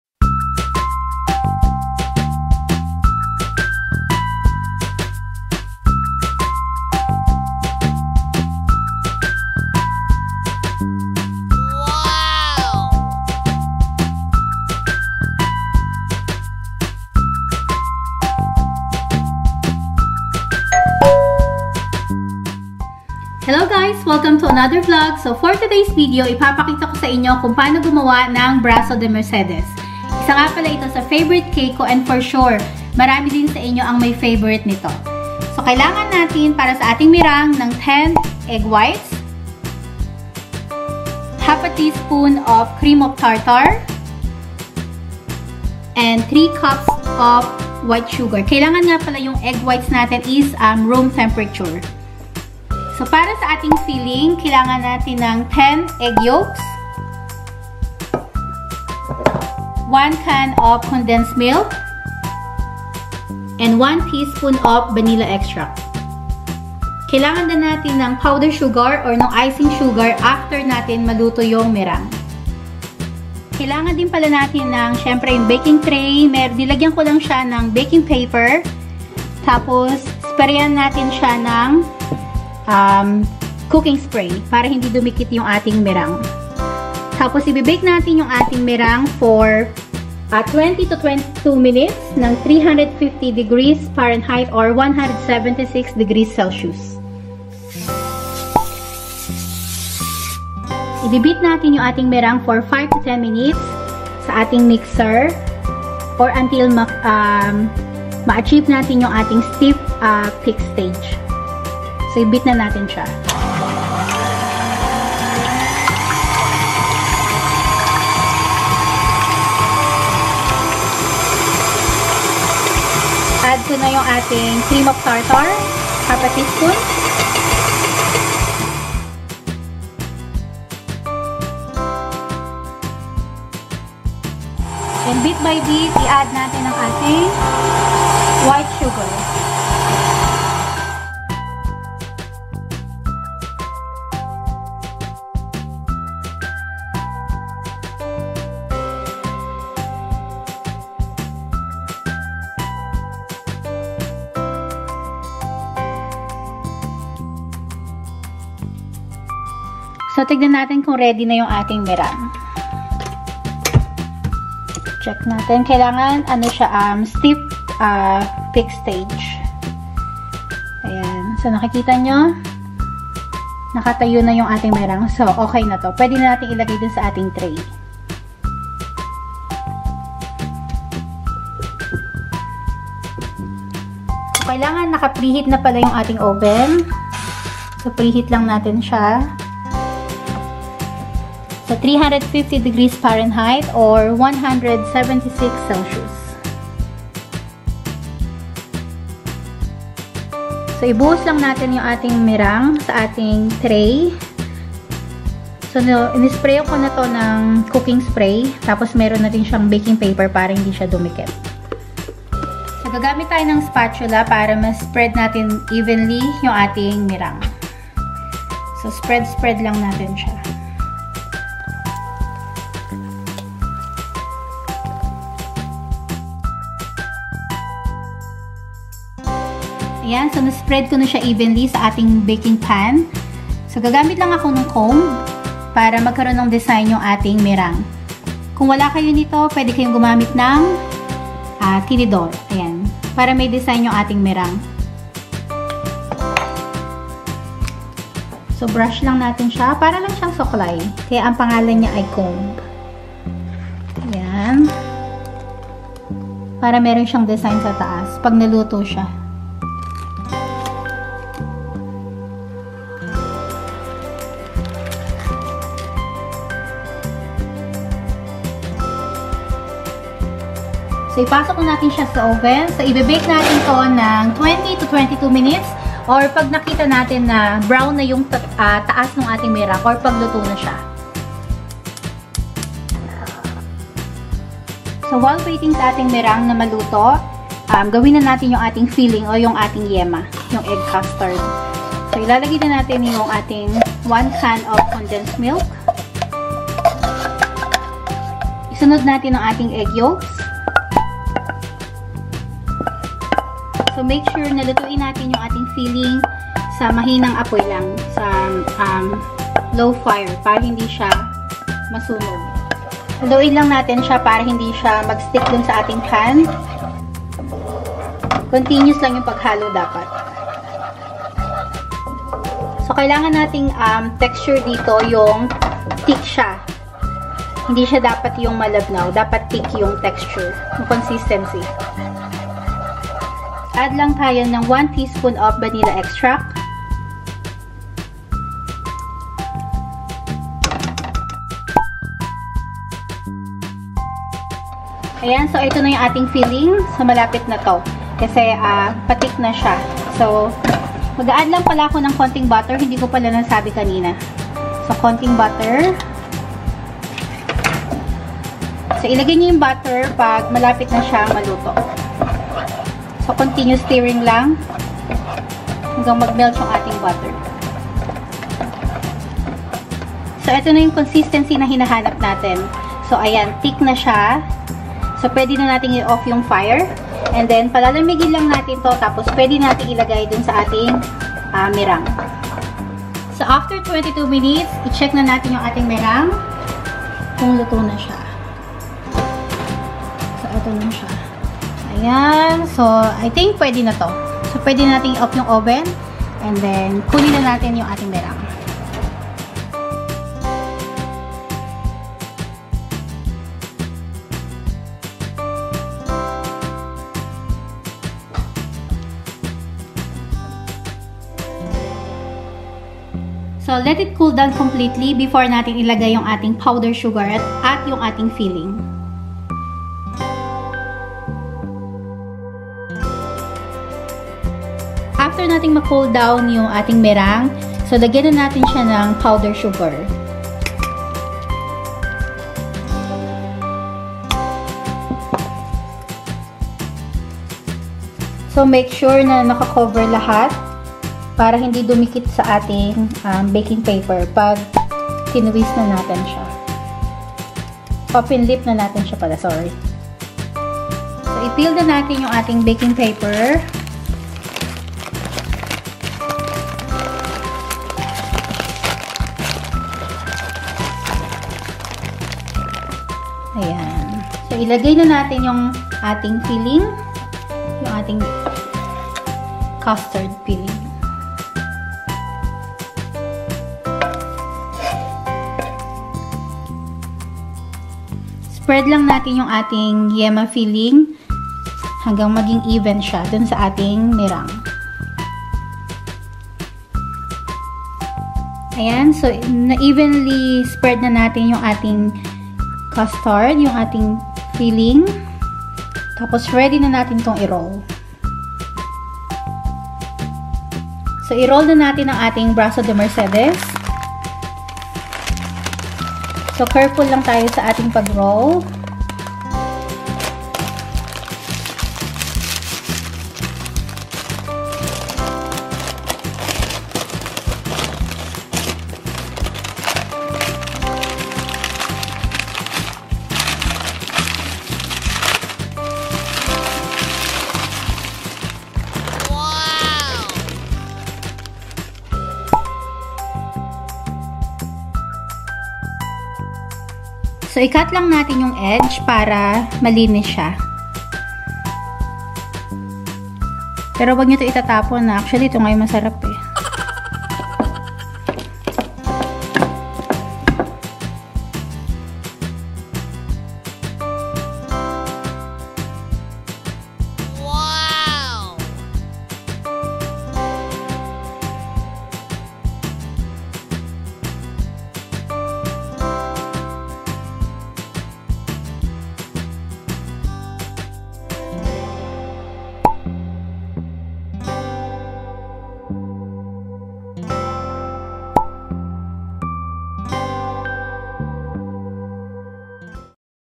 BRAZO DE MERCEDES RECIPE. VLOG # 36
Hello everyone, Here’s the recipe of Brazo De Mercedes. A popular dessert in the Philippines. Must try, delicious 😋

Ingredients:
For the Meringue
10 egg whites (room temperature)
3/4 cup white sugar
1/2 tsp cream of tartar
For the filling (egg custard)
10 egg yolks
1 can condensed milk (any brand)
1 tsp vanilla extract
2 tbsp butter
We will also be needing Powder/Icing Sugar & Oil spray

Bake at Preheated oven for 20-22 mins at 350 °F or 176 °C. I used 13 in X 9 in baking pan. Watch the video for the full instructions. Any question?

📍TIPS
✏️ The meringue bowl must be grease free, it means NO oil, butter or else we can’t achieve good meringue.
✏️ Grease all sides of the pan generously. So the meringue will not stick too much on the baking paper, it’s more easier to peel, but be careful removing the baking paper. Breaking the meringue? No worries you will cover it with egg custard.
✏️ Separate the eggs one by one  in the separate small bowl, then put it in the mixing bowl. This way, you will not risk the whole batch of egg whites when a yolk accidentally taint in it. Egg yolk in the egg whites? BIG NO NO.
✏️ Be gentle while rolling the Brazo, Meringue is fragile.
✏️Use room temperature eggs
✏️You can leave the meringue in the oven after baking (oven off) for 5- 7  mins, give it a time to atleast adjust on the temperature outside.
✏️ Do not over beat the meringue

Melvin & Joyce
Melbourne Australia